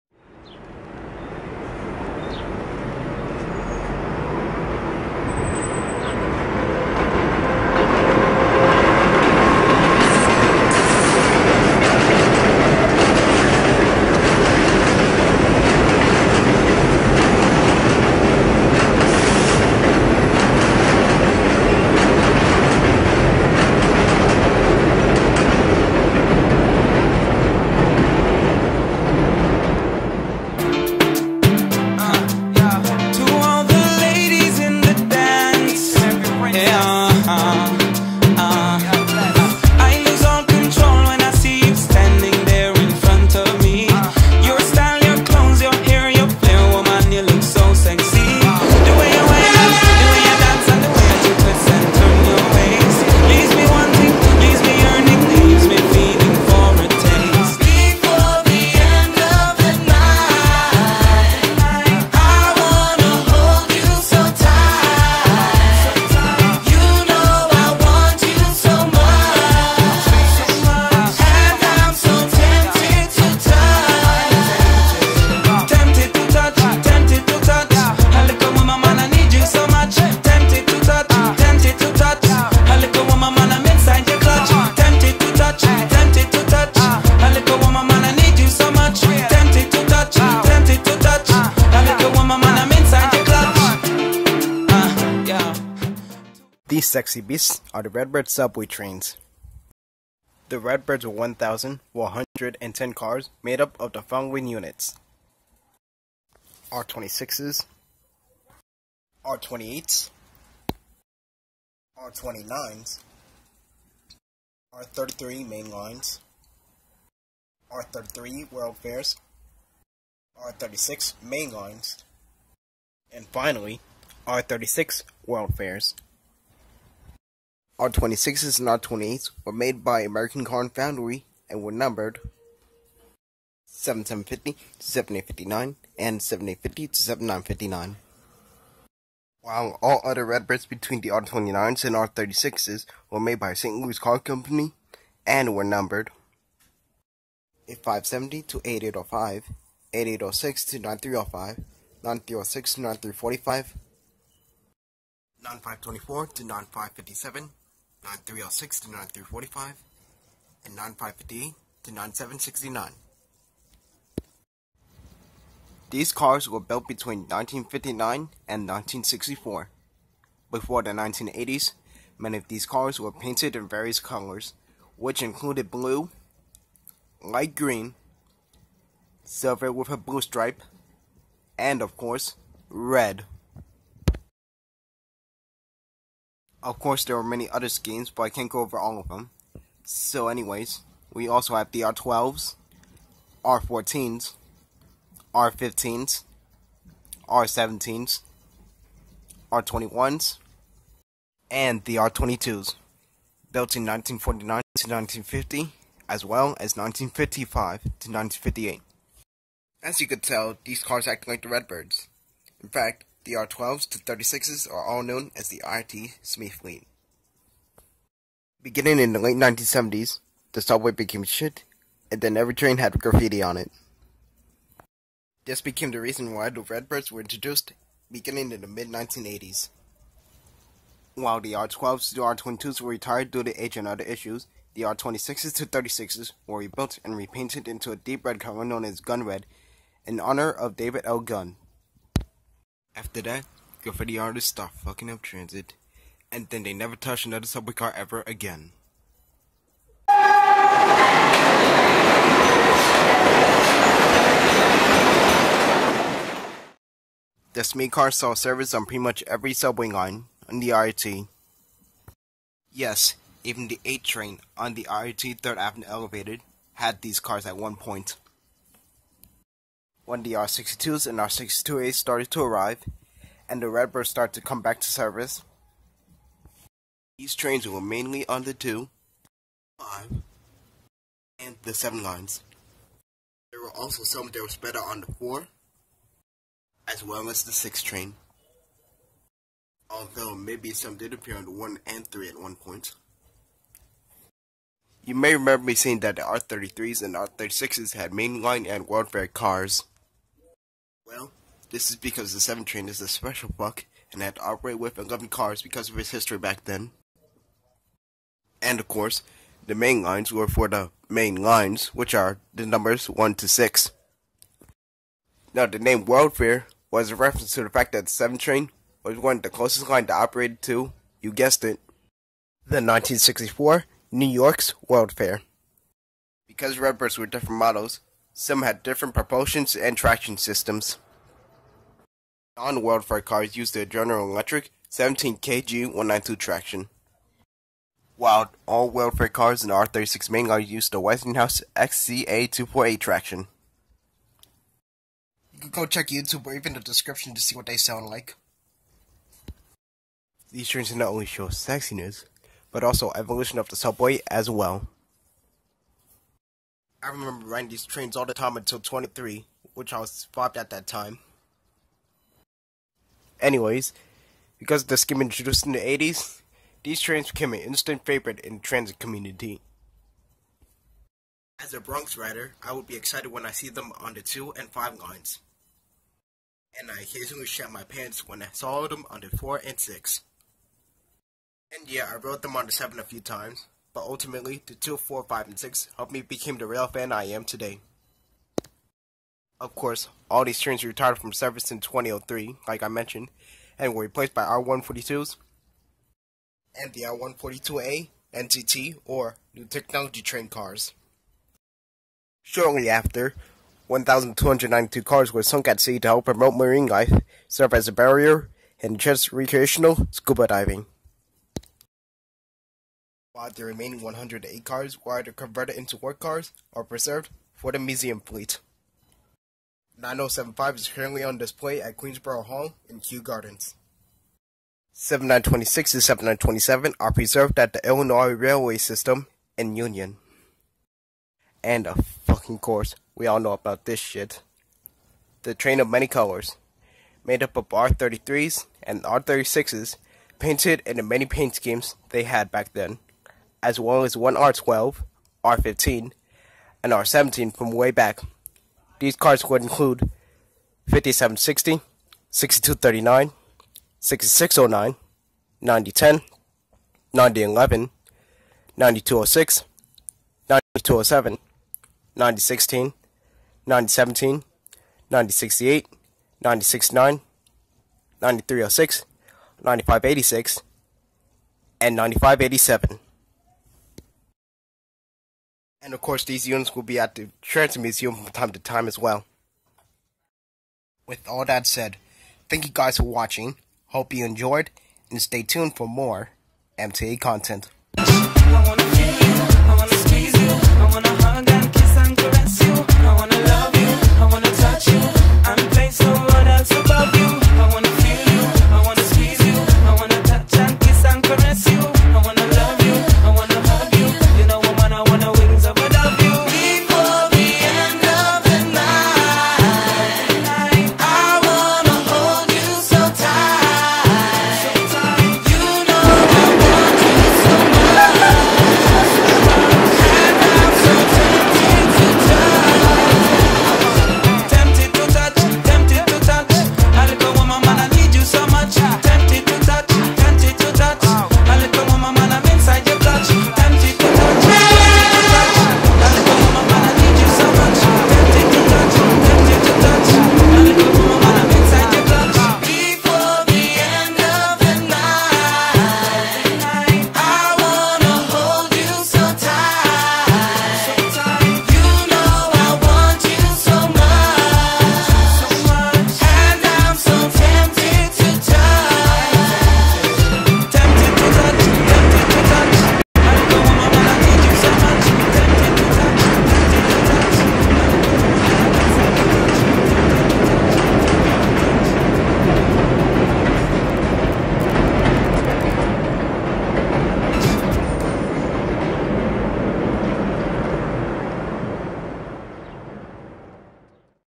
IRT Redbird subway train review
First review of New York City Transit: NYC Redbird trains. These cars helped me become the railfan I am today and also can relate to almost every other railfan.

Keep in mind that this is the first subway review I ever did so of course there may be a few details that are either off or not included. As for the theme song, I'm not going to defend it 100%, but the lyrics represent what is/was on my mind when I think of Redbirds. It's definitely not the worst choice however.